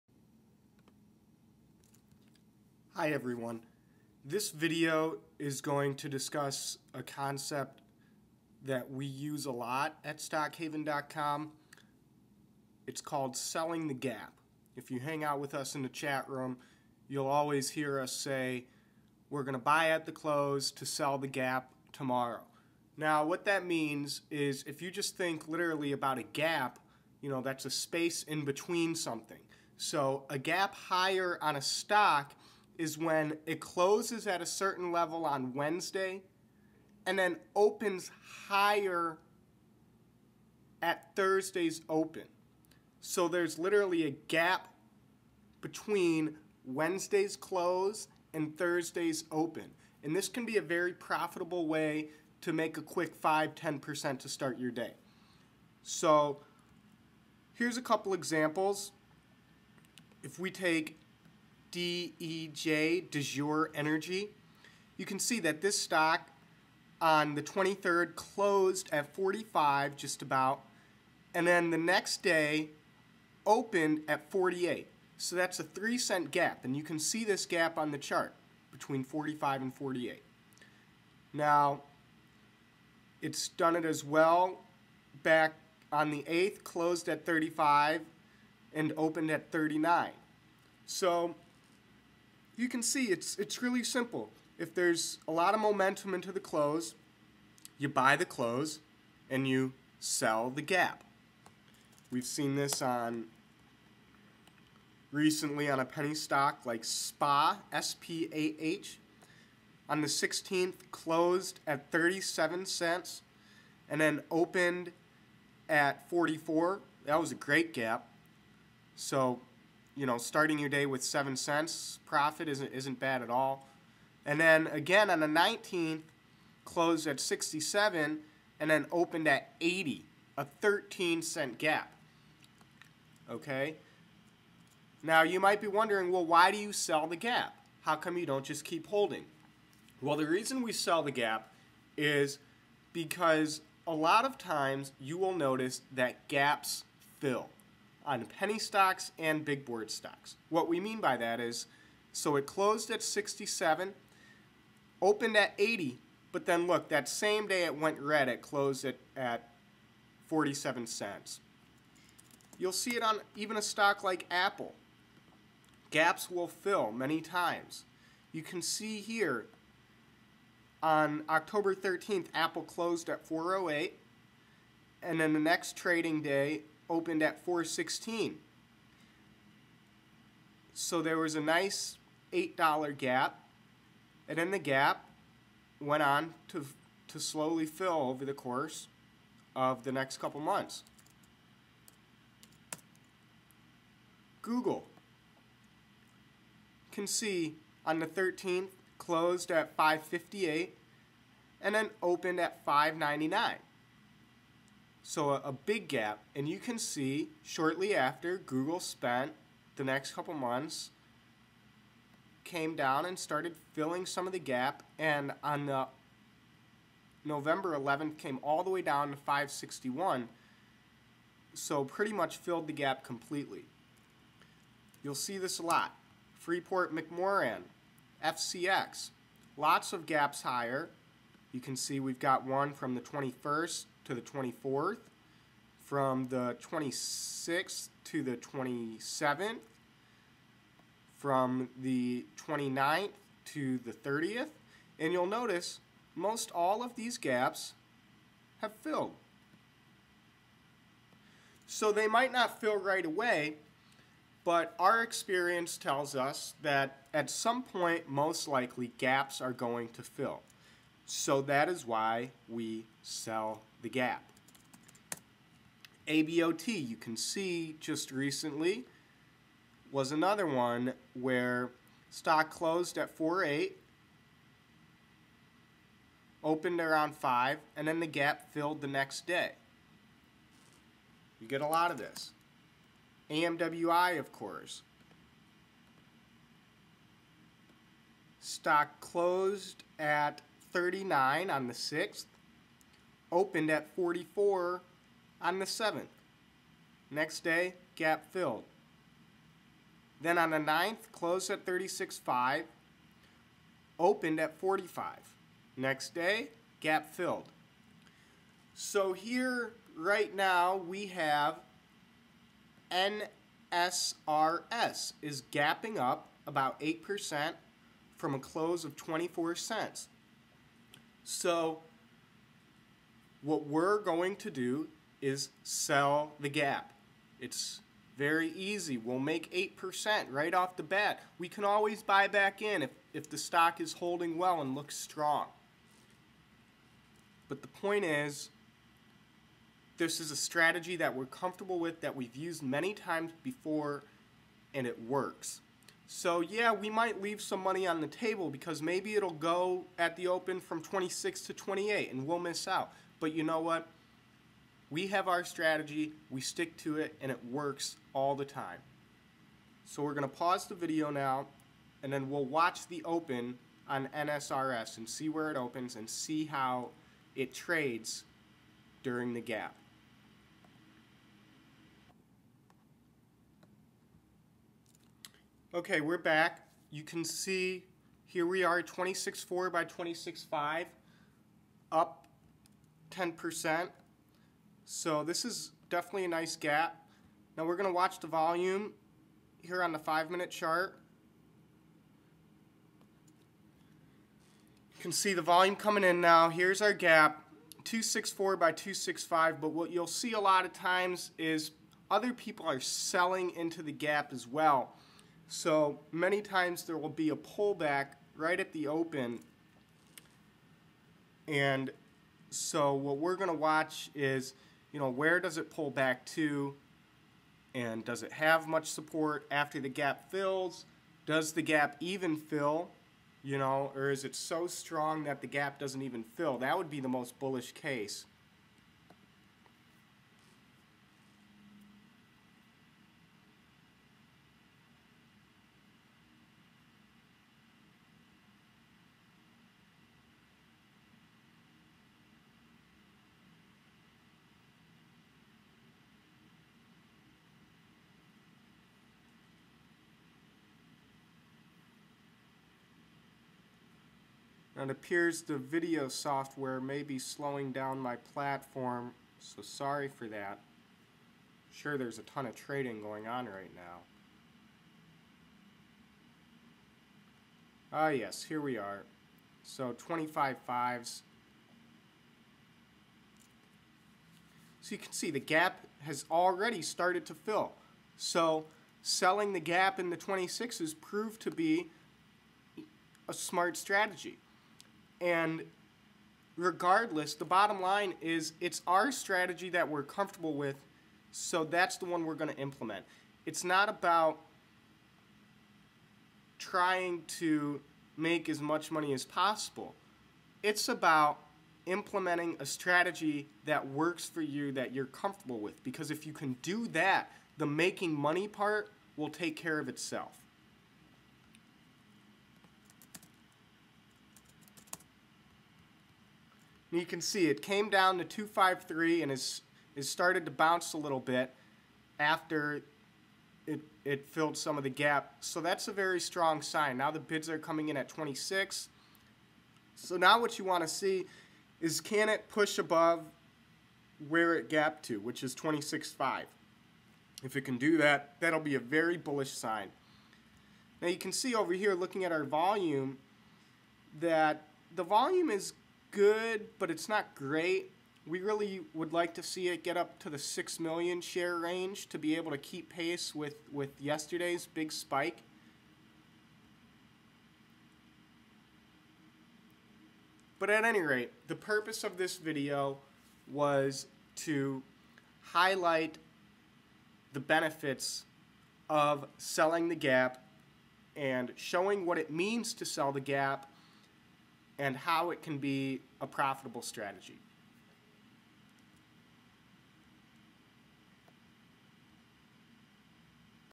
"Selling the gap" -- How to sell the gap for profits on stocks
This video explains the trading term "selling the gap" and the strategy behind it. Selling the gap is a very profitable strategy that we use at stockhaven.com to make quick and easy profits on a regular basis, this video will explain how it's done.